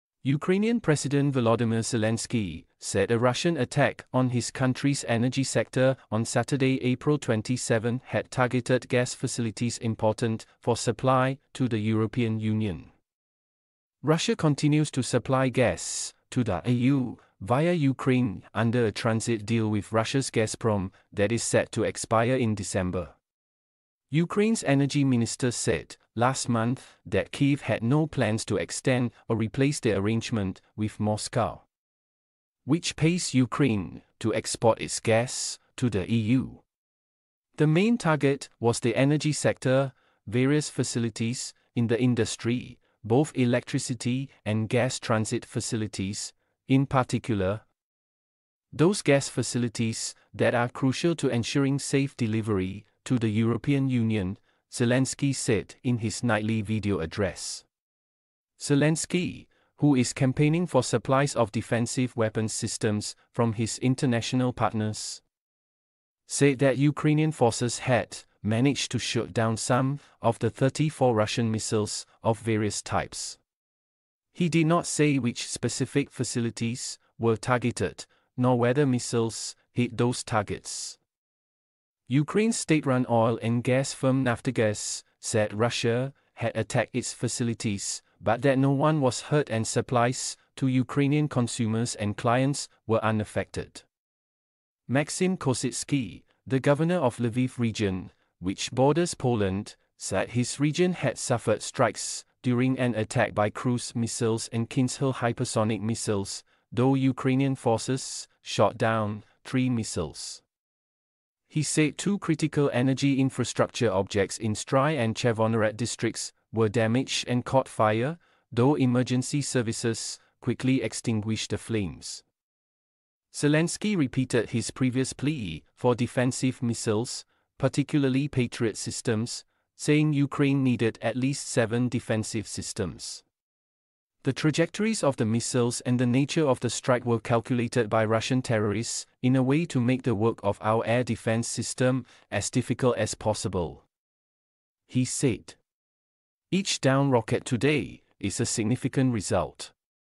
Zelenskyy says Russia targeted gas facilities that secure EU supply
💗📌 Singapore